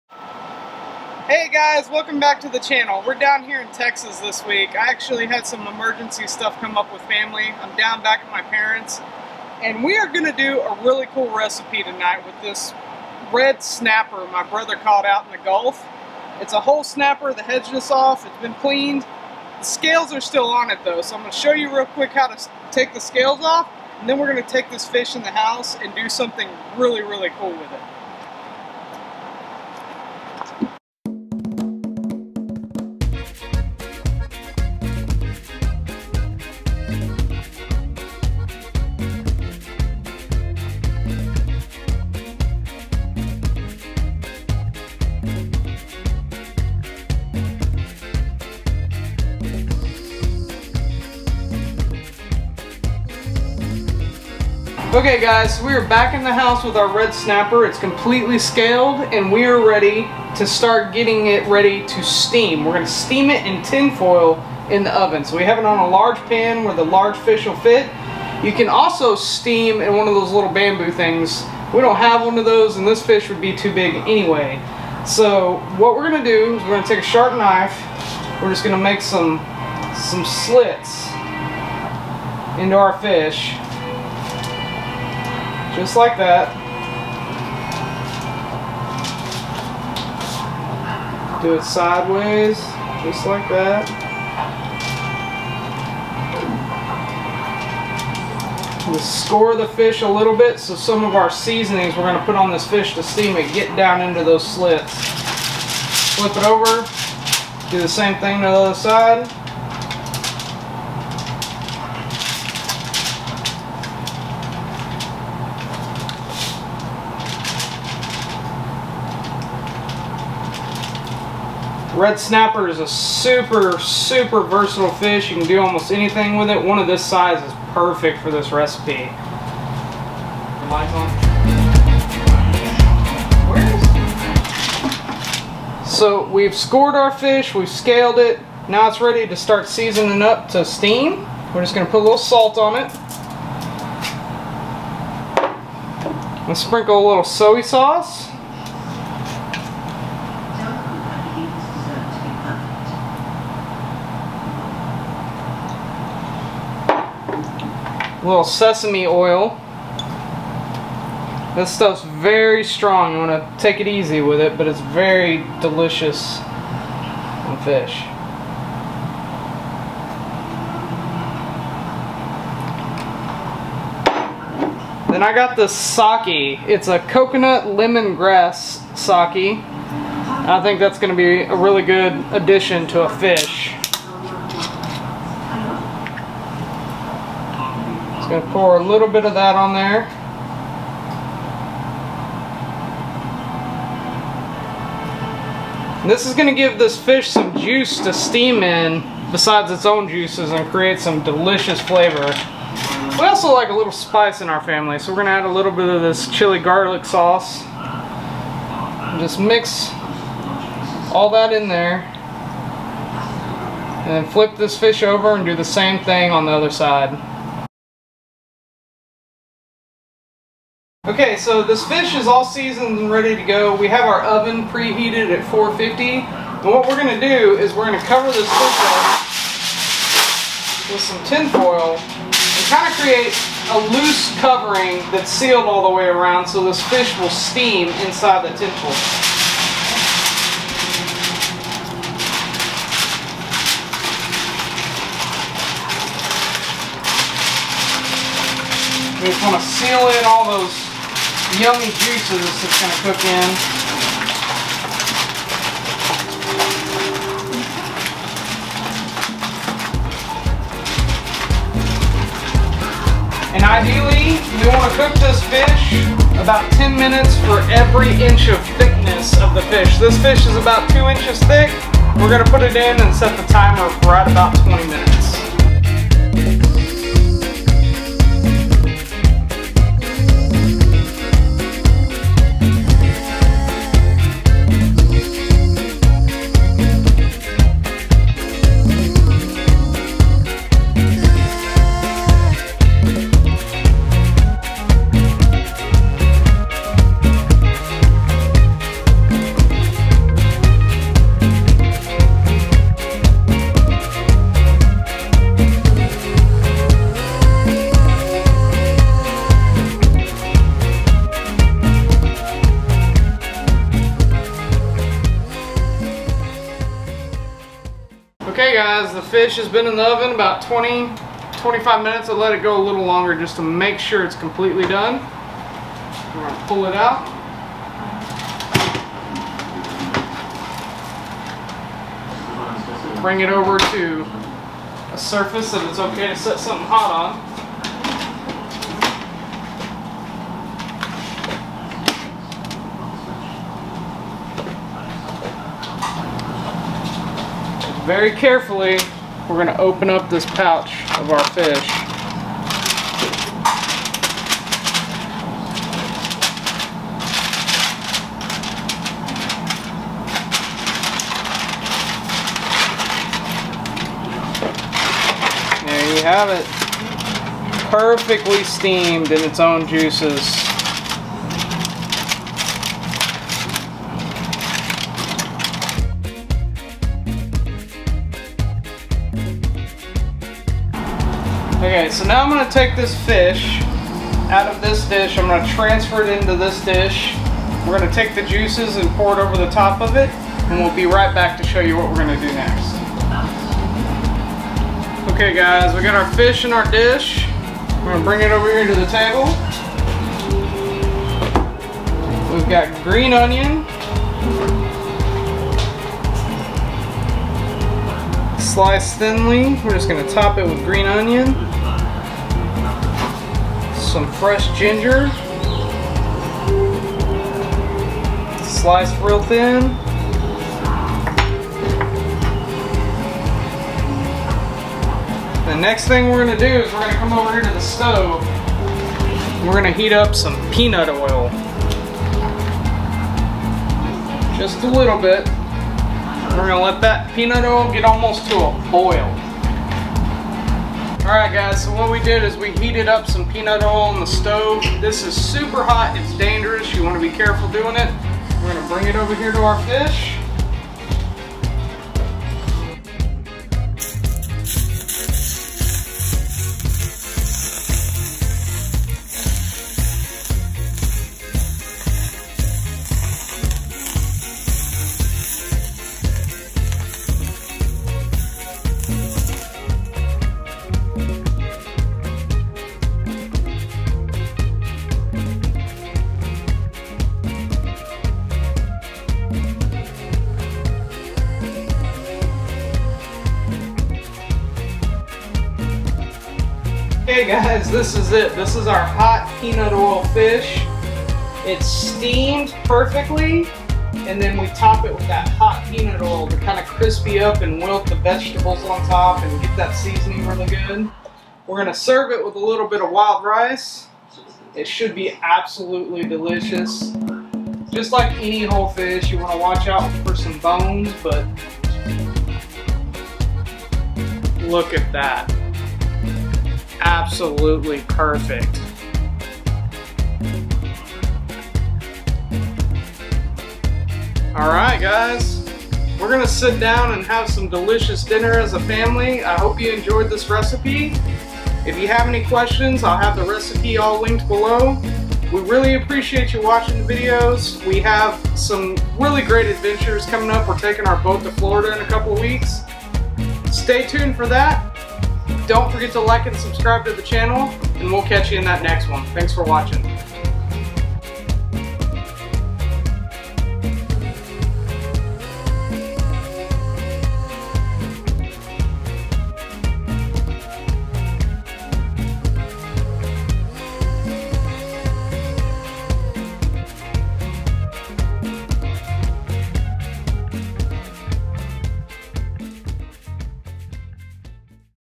Fresh Steamed Gulf Red Snapper with HOT PEANUT OIL POUR OVER
In this video I steam some fresh Gulf red snapper and finish with a hot peanut oil pour over. This recipe is super simple and delicious and really bring our the best texture and flavor of fresh fish.

Scale and score the fish skin the season with salt, sake, sesame oil, soy sauce and some chili garlic sauce
Steam in the oven on 450 wrapped in tin foil for 10 minutes per inch of thickness. When done top fish with fresh sliced ginger and green onion then carefully heat peanut oil up to steaming point then VERY CAREFULLY pour oil over the steamed fish and fresh veggies. The hot oil with wilt the onion and ginger and crisp the skin a bit and just brings out the most spectacular flavor.